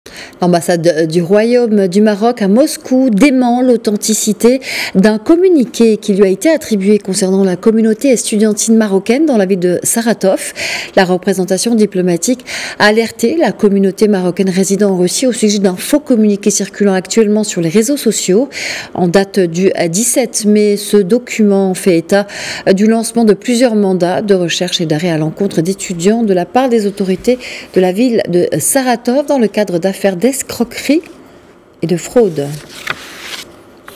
Etudiants de Saratov : l'ambassade du Maroc alerte sur un faux document
L'ambassade du Maroc à Moscou dément l’authenticité d’un communiqué sur la communauté estudiantine de Saratov